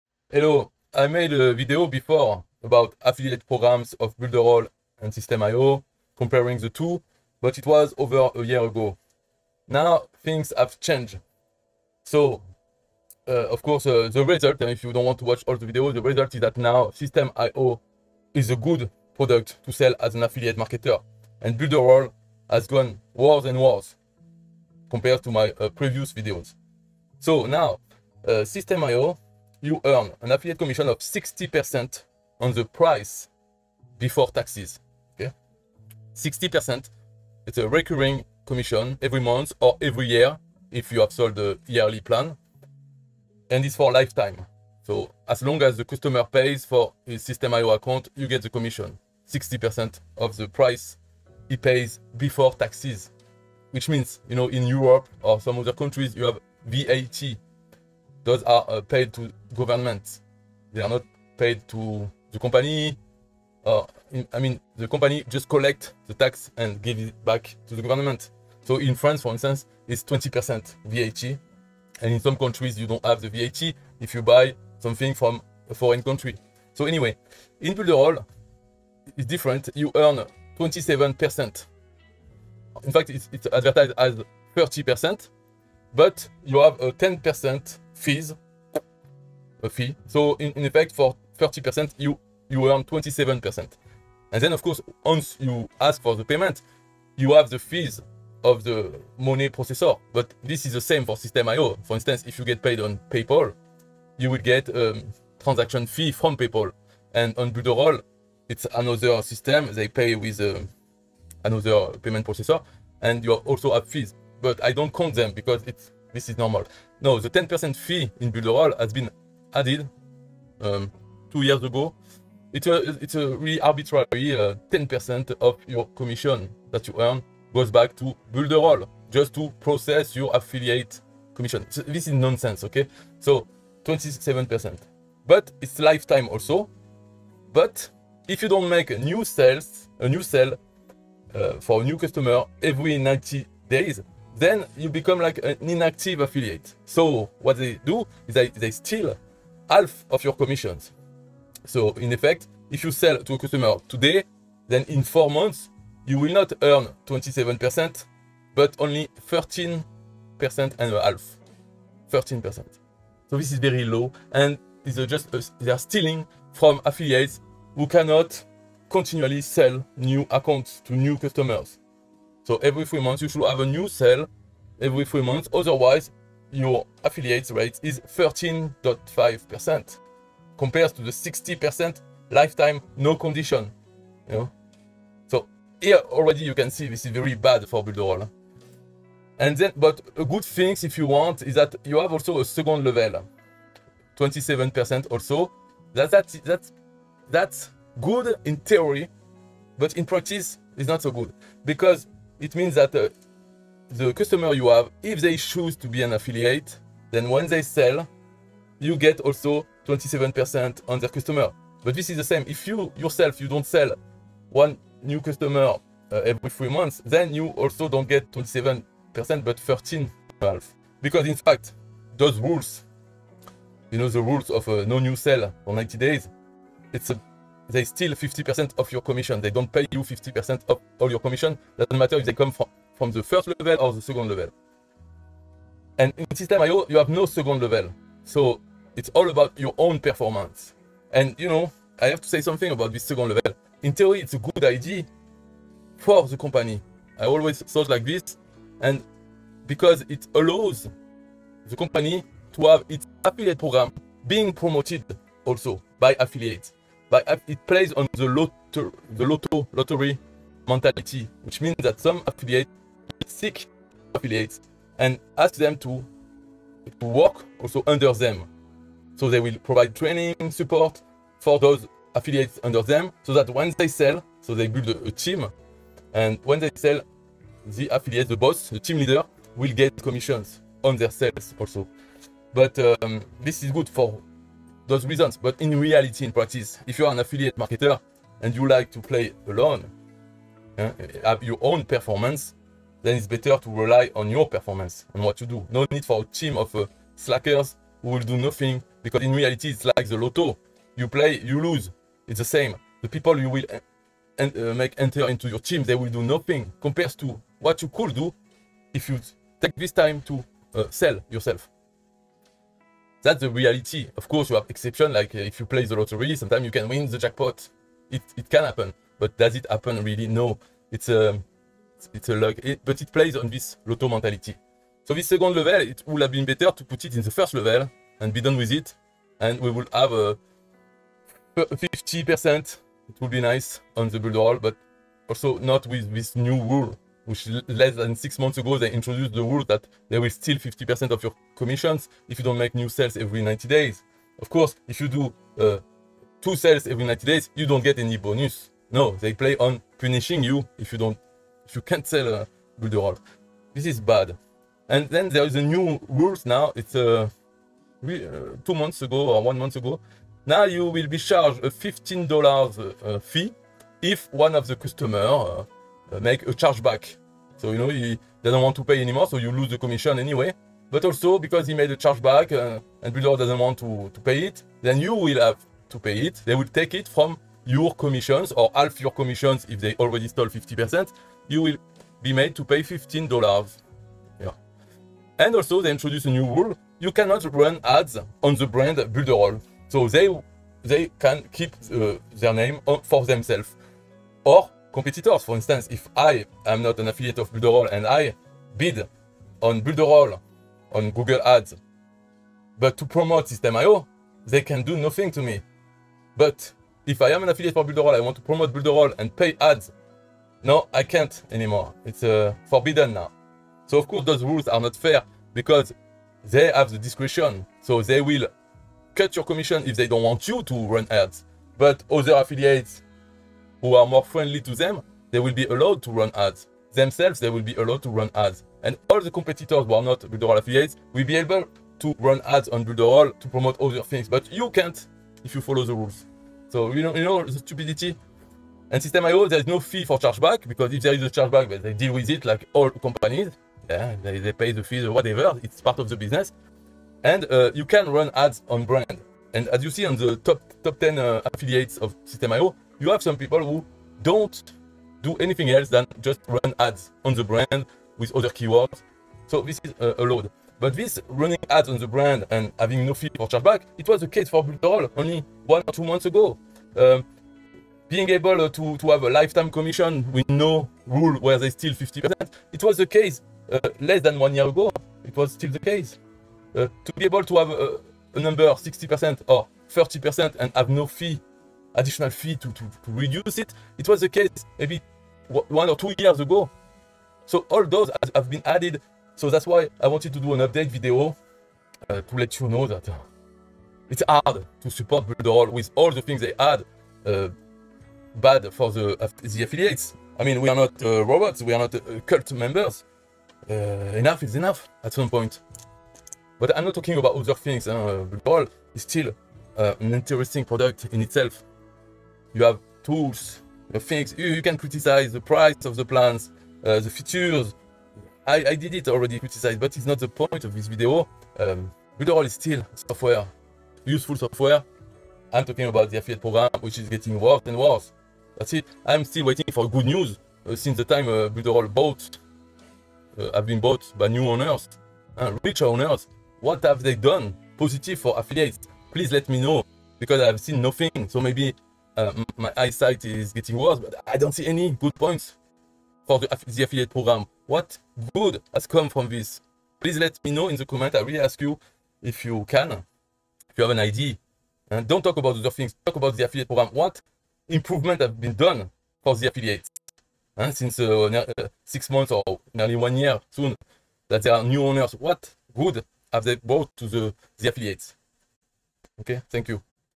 Builderall vs. SystemeIO affiliates programs comparison
... In this video, we compare the affiliate programs Builderall and SystemeIO. We look at the advantages and disadvantages of each program, as well as their features and tools. If you're considering becoming an affiliate in online marketing, this video will help you make an informed decision. We review the commissions offered, the training resources available and the marketing tools offered by each of these programs. Watch this video to learn more about Builderall and SystemeIO and find out which affiliate program best suits your needs and goals.

Builderall is a registered trademark and "Build'N'Roll" is a participant in the Builderall Affiliate Program, designed to provide a means for sites to earn advertising fees by advertising and linking to Builderall.